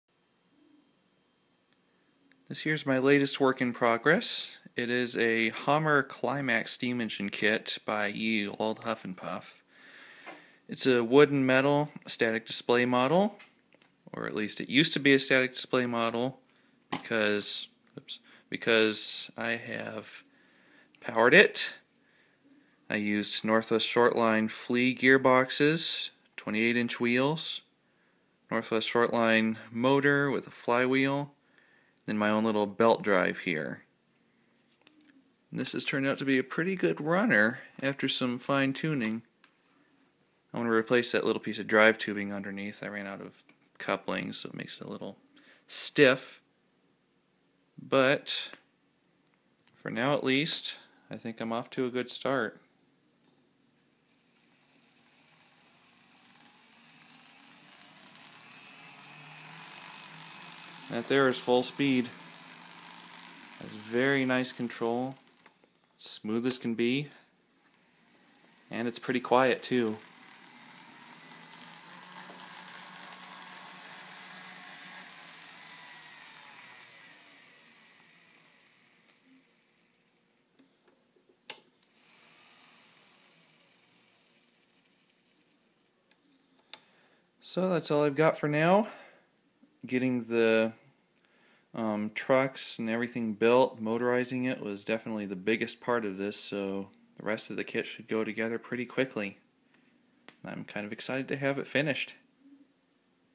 Powered Climax steam engine project!
Another interesting project! This time, I've powered a Hommer Climax steam engine from Ye Olde Huff 'N' Puff. This kit was not made to be powered by any means, so it took some creativity to do it. I was told powering this kit was basically impossible, but I must have done something terribly wrong because it works!

It's powered mostly with NWSL parts, and the truck sideframes came from Mark Rollins, who is the only one I know of anywhere making these parts.